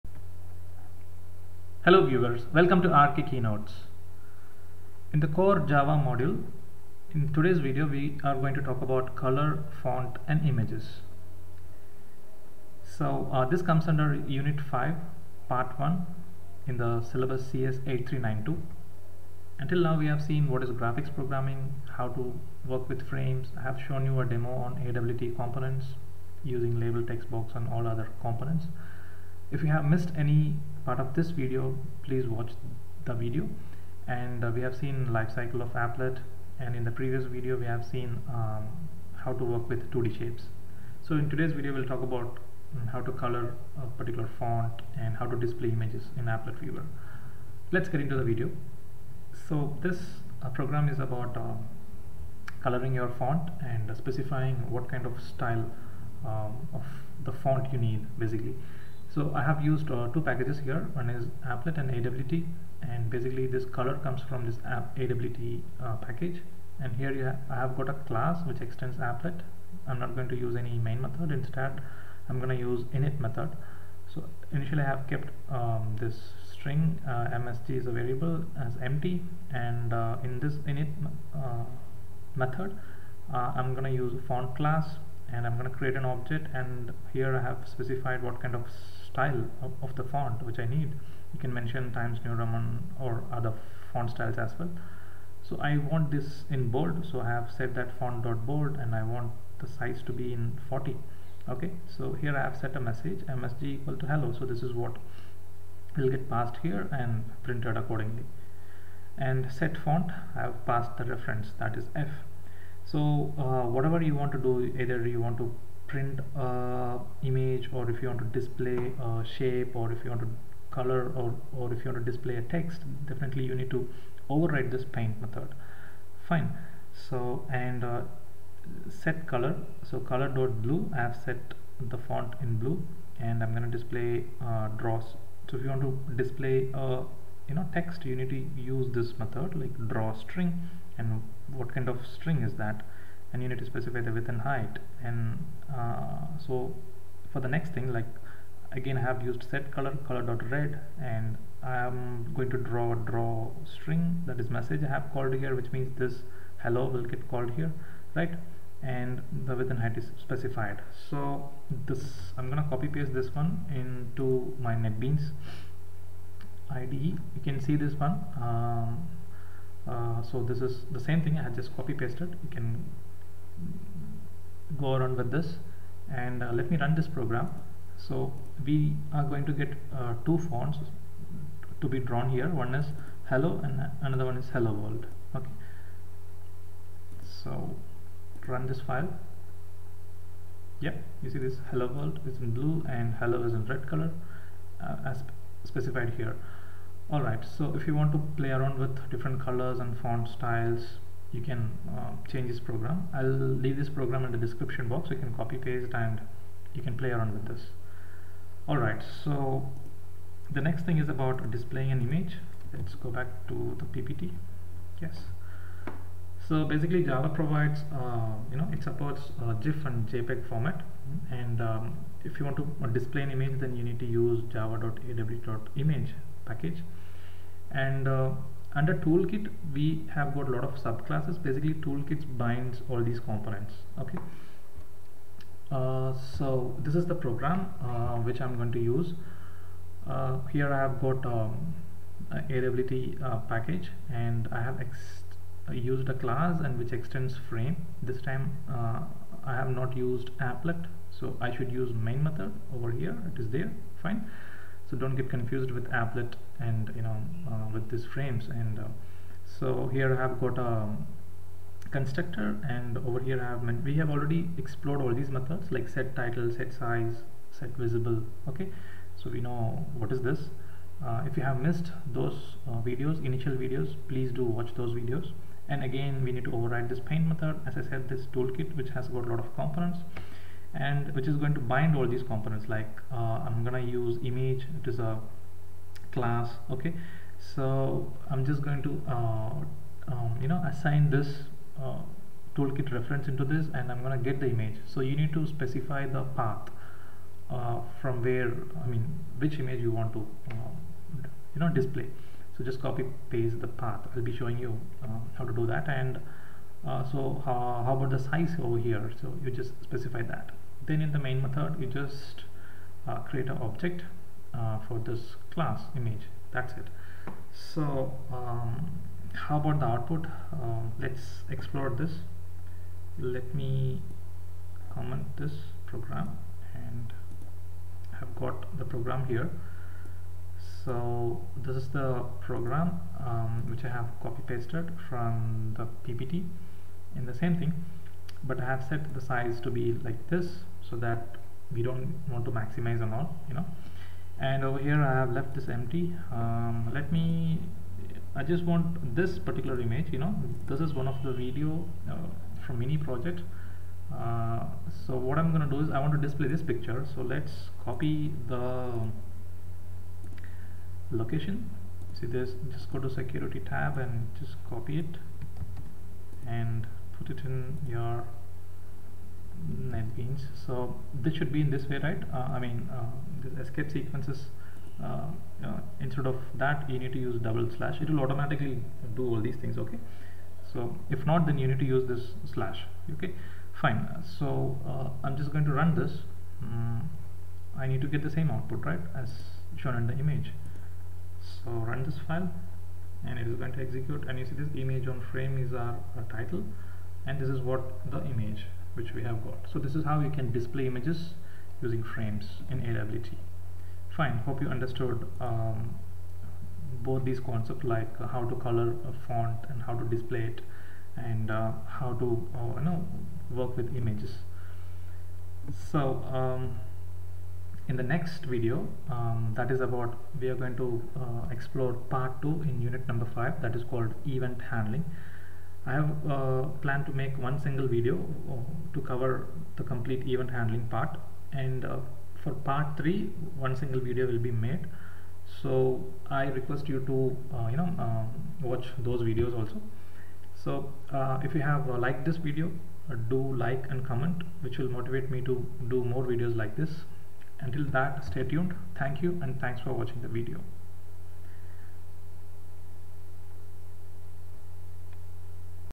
(Unit 5 - 06) How to Display Image using AWT | How to Color Font using Applet | Demo | RK Keynotes
How to Display Image using AWT?
How to Color Font using Applet?
Toolkit?
Image?
DrawString?
Font?

Source Code
//Image

 import java.awt.*;
public class ColorFontImage extends Frame
public ColorFontImage()
{ setTitle("Image on Frame");
setSize(1400,1400);
setVisible(true);
public void paint(Graphics g)
{ Toolkit tk = Toolkit.getDefaultToolkit();
Image img = tk.getImage("C:\\Users\\Ravikumar\\Desktop\\maxresdefault.jpg");
g.drawImage(img,100,100,this);
public static void main(String args[]){
new ColorFontImage();//anonymous obj to access constructor

//Font_Color
import java.applet.*;
import java.awt.*;
//import java.awt.Color;

public class ColorFontImage extends Applet{
String msg="";
public void init()
Font f;
f= new Font("Monotype Corsiva", Font.BOLD,40);
msg="Hello";
setFont(f);
g.setColor(Color.blue); //set color to blue
g.drawString("Hello World...",30,180); //print hello world

g.setColor(Color.red);
g.drawString(msg, 10,50);

Useful PlayList's

Regards,
Ravikumar R N